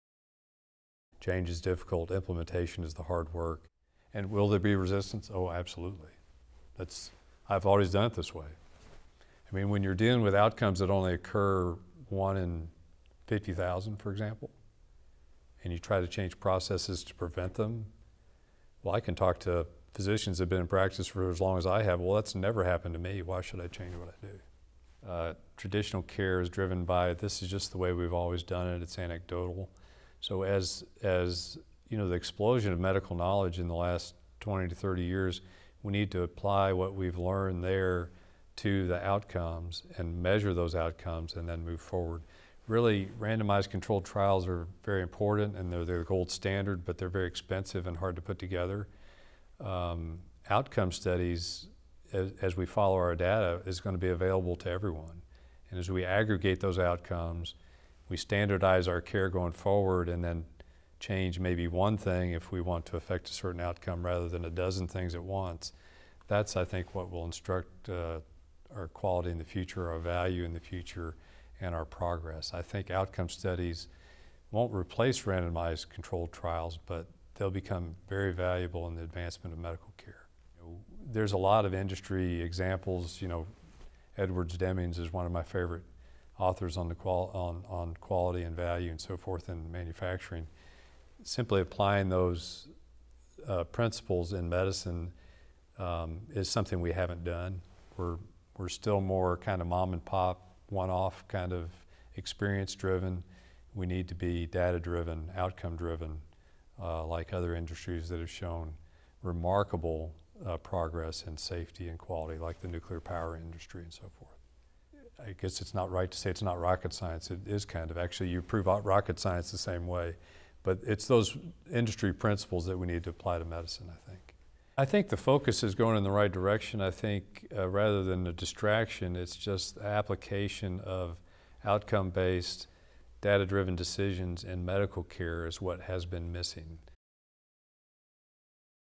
Dalton: Change is Hard, but Other Industries Can Show the Way
John Dalton, MD, is the Chief Quality Officer of PhyMed, a data-driven healthcare group based in Nashville, Tennessee. He stopped by the studio at Healthcare Analytics News to discuss the importance of moving medicine to a data-driven, outcome-focused industry, and the cultural difficulties that go along with that:

“Change is difficult, implementation is the hard work, and will there be resistance? Oh, absolutely.

I mean, when you're dealing with outcomes that only occur 1 in 50,000 for example and you try to change processes to prevent them…I can talk to physicians that have been in practice for as long as I have: ‘Well that's never happened to me why should I change what I do?’ Traditional care is driven by ‘This is just the way we've always done it.’ It's anecdotal, so with the explosion of medical knowledge in the last 20 to 30 years we need to apply what we've learned there to the outcomes and measure those outcomes and then move forward.

Randomized control trials are very important, and they're their gold standard, but they're very expensive and hard to put together. Outcome studies, as we follow our data, are going to be
available to everyone. As we aggregate those outcomes, we standardize our care going forward and then change maybe one thing if we want to effect a certain outcome, rather than a dozen things at once. I think that’s what will instruct our quality in the future…I think outcome studies won't replace randomized controlled trials, but they'll become very valuable in the advancement of medical care.

There's a lot of industry examples. Edwards Deming is one of my favorite authors on quality and value and so forth in manufacturing: simply applying those principles in medicine is something that we haven't done. We're still kind of a mom-and-pop, one-off, experience-driven [industry]. We need to be data-driven and outcome driven like other industries that have shown remarkable progress in safety and quality, like the nuclear power industry.

I guess it's not right to say it's not rocket science, it is kind of, actually…you prove out rocket science the same way. It's those industry principles that we need to apply to medicine. I think the focus is going in the right direction. Rather than a distraction, application of outcome-based-and-driven decisions is what medical care has been missing.”